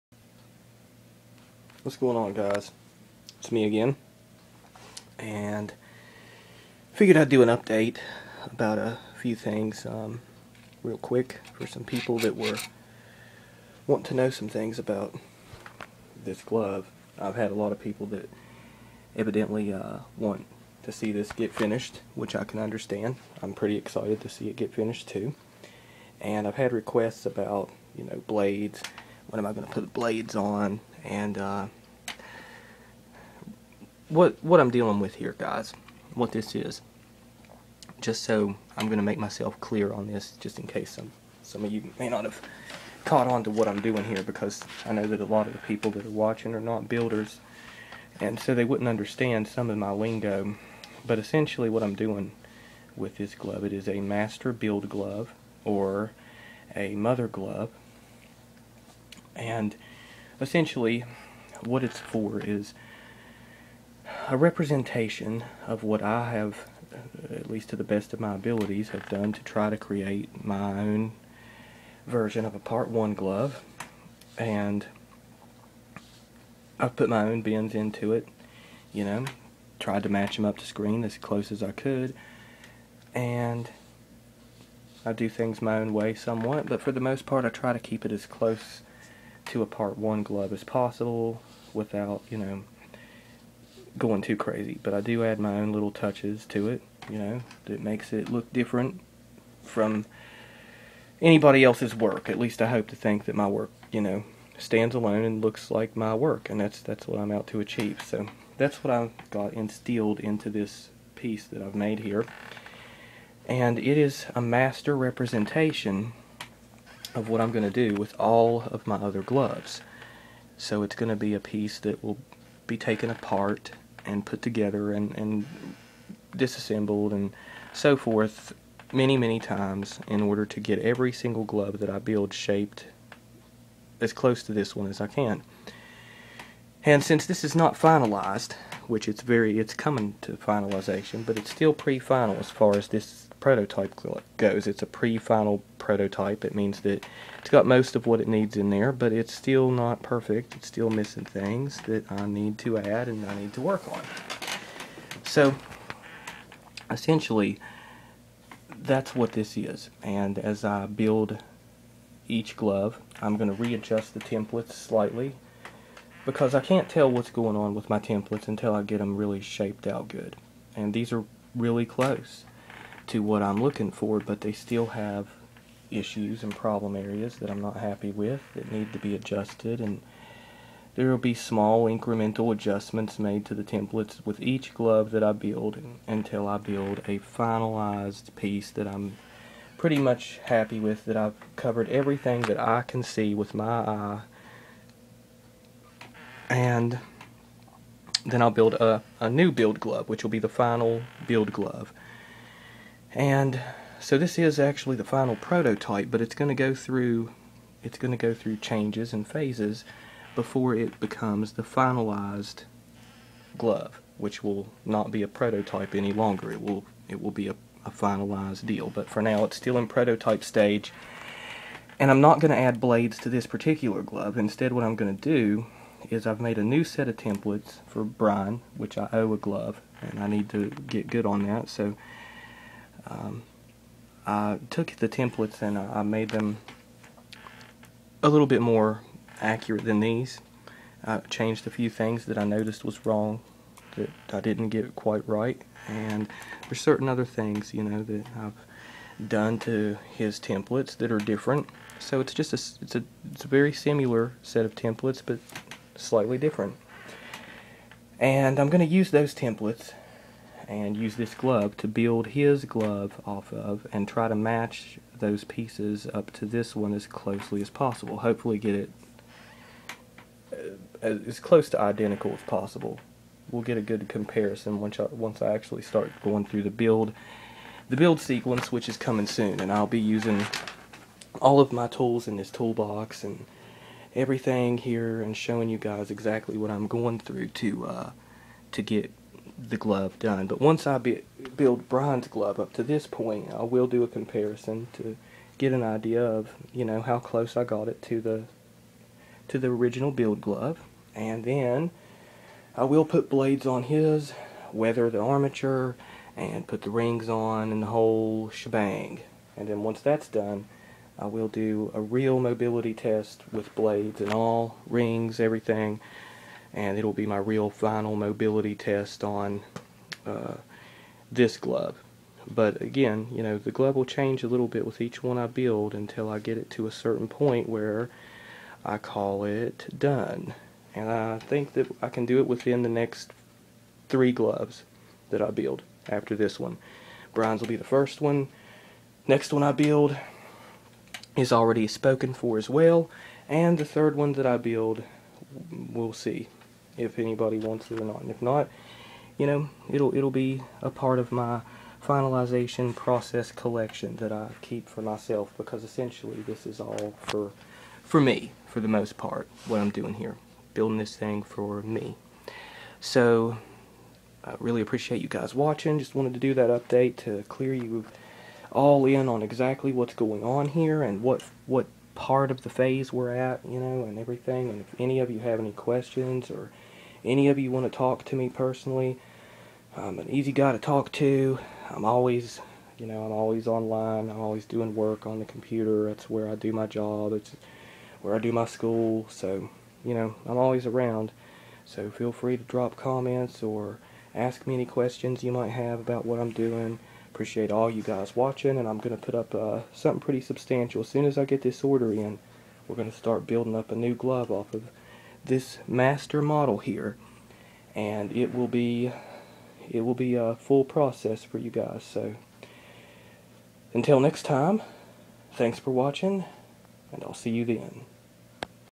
Finalization of My Part One Glove Part 14 5/30/2013
This is a series of videos marking my progress with the Part One Glove.